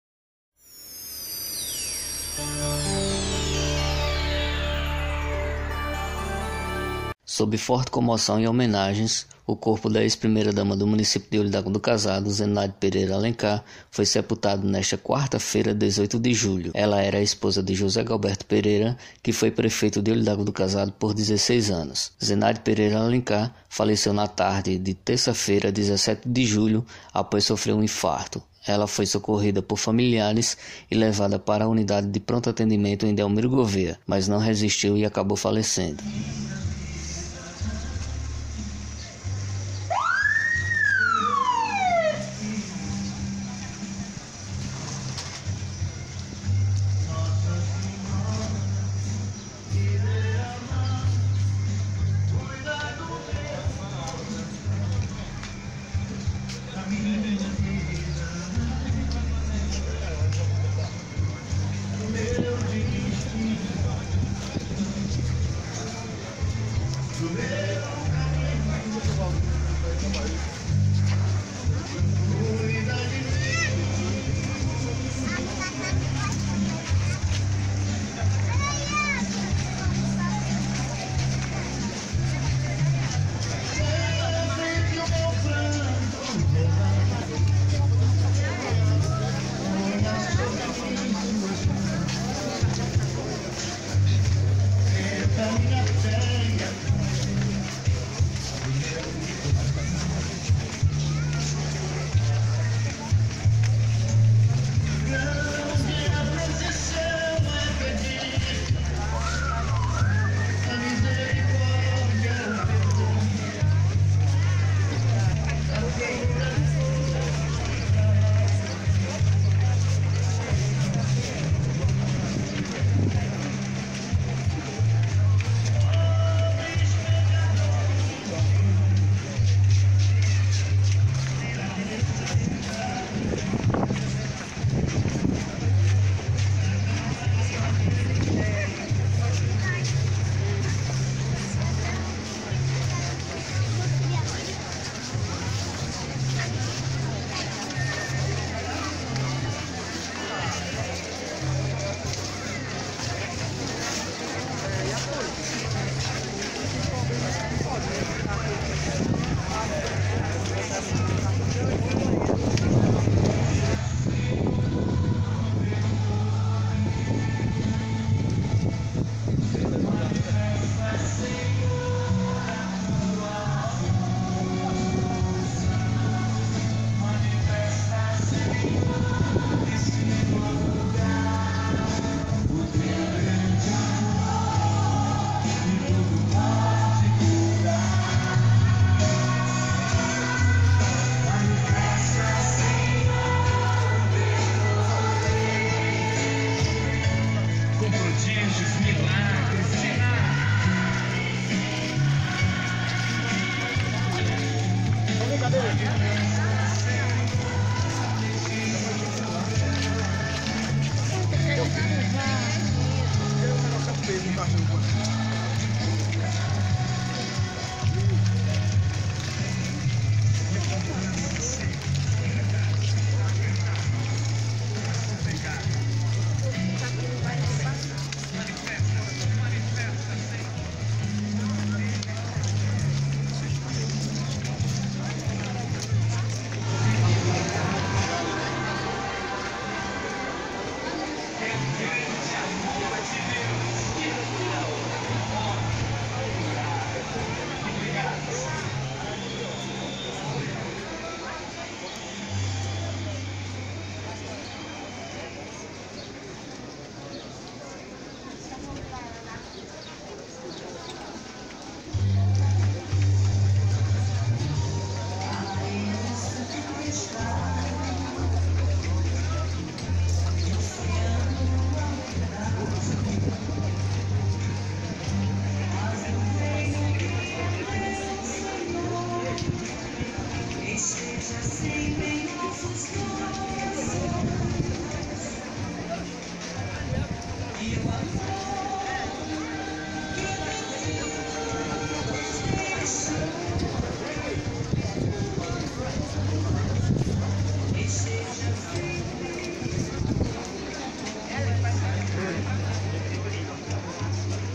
Corpo da ex-primeira-dama, Zenaide Pereira Alencar é sepultado em Olho D'Água do Casado
Corpo da ex-primeira-dama, Zenaide Pereira Alencar é sepultado nesta quarta-feira, 18 de julho na cidade de Olho D'Água do Casado, sertão de Alagoas.